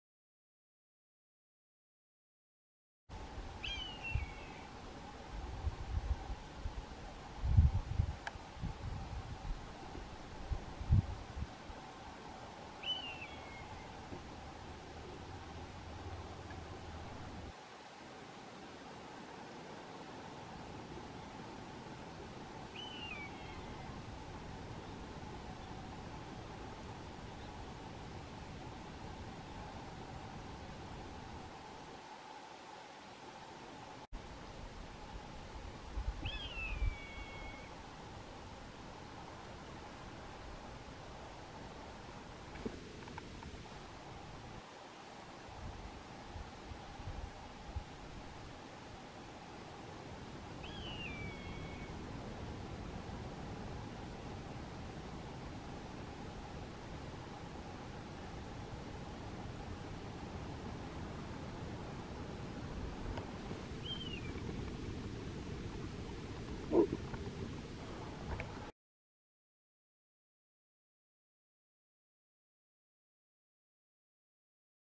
🦅👀ノスリが鳴きながら飛翔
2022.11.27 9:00
ノスリが鳴きながら飛翔していた。